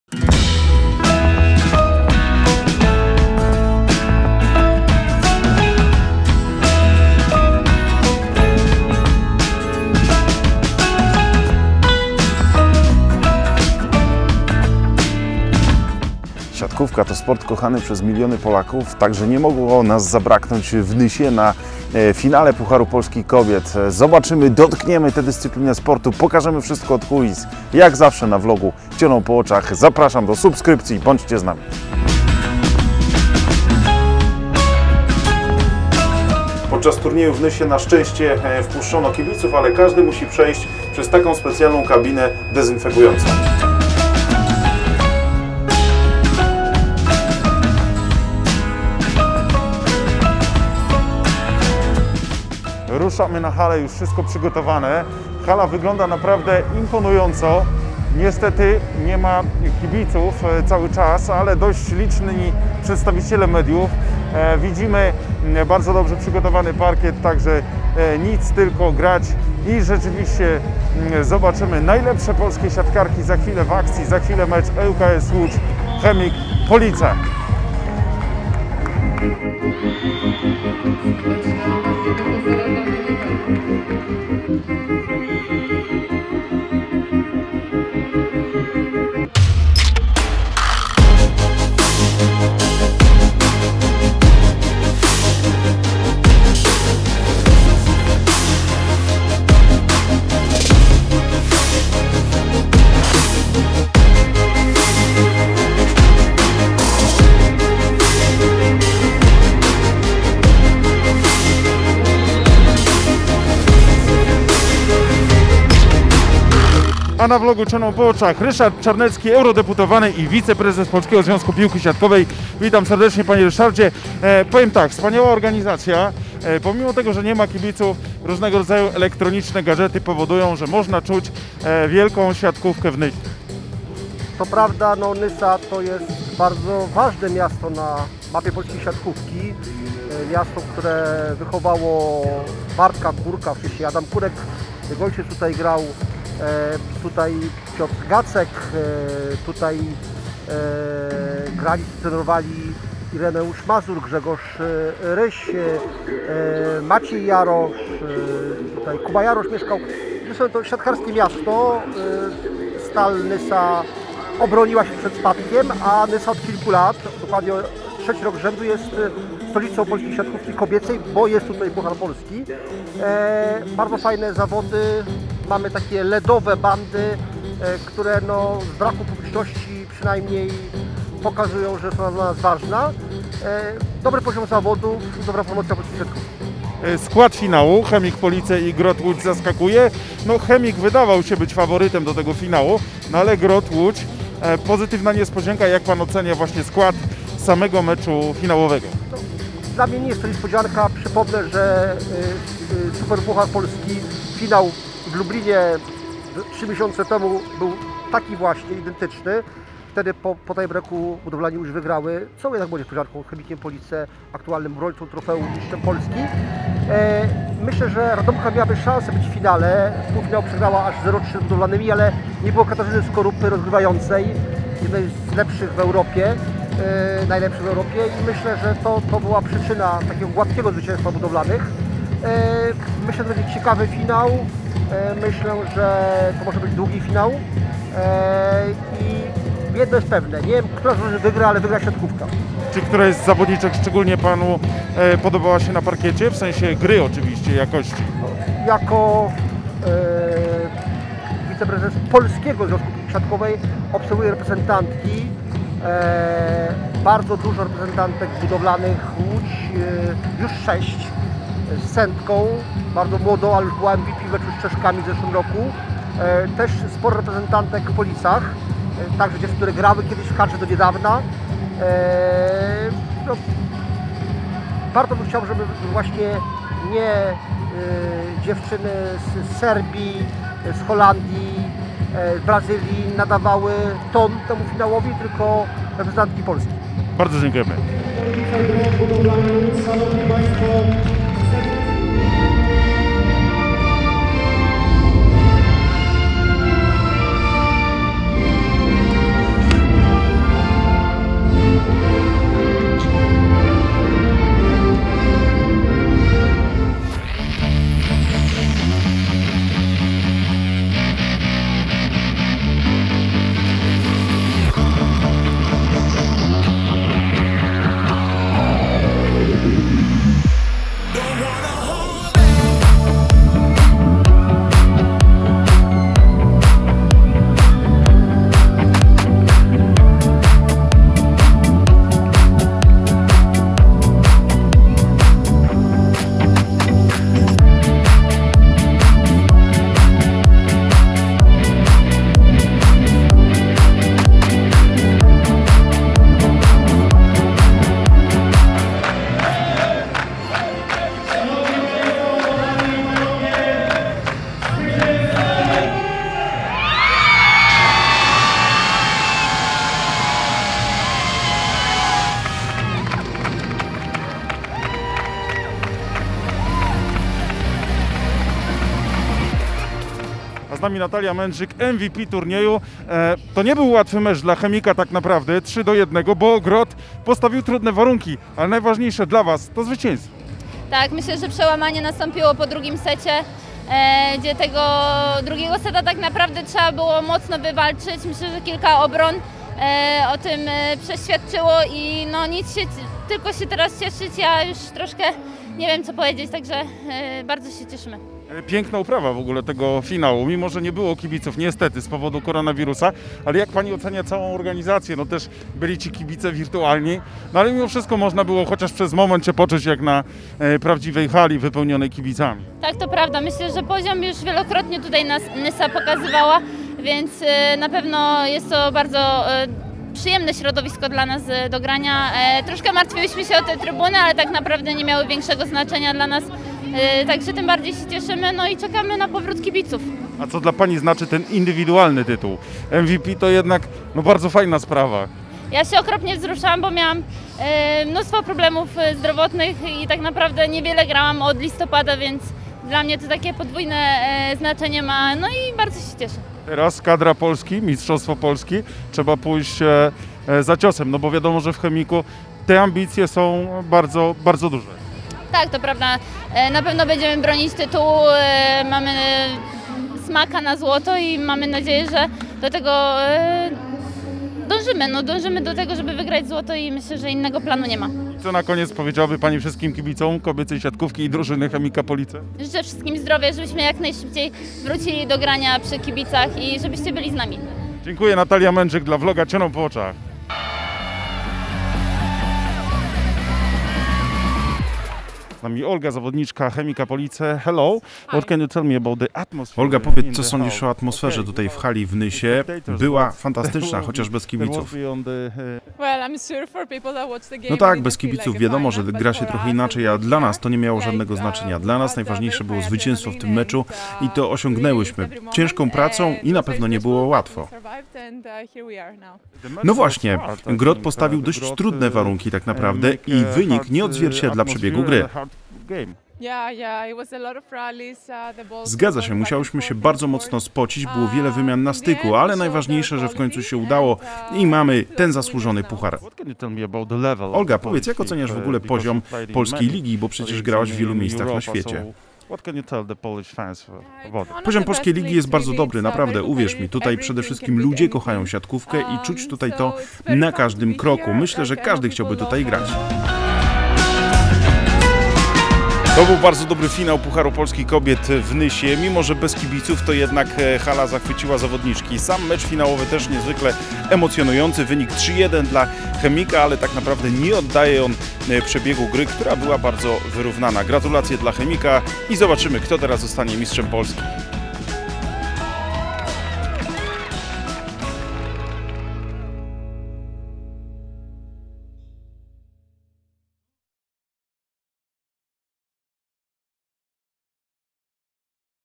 FINAŁ PUCHARU POLSKI KOBIET II CHEMIK POLICE - GROT BUDOWLANI ŁÓDŹ 3-1 II KULISY
Zapraszamy na ekscytujący materiał w którym znajdziecie kulisy, gorące wypowiedzi i smaczki jakie nie znajdziecie nigdzie indziej z finałowego turnieju o puchar Polski Kobiet w Nysie. W materiale między innymi wywiady z Natalią Mędrzyk - MVP Turnieju, zawodniczką Chemika Police oraz Ryszardem Czarneckim wiceprezezem PZPS. Zapraszamy!!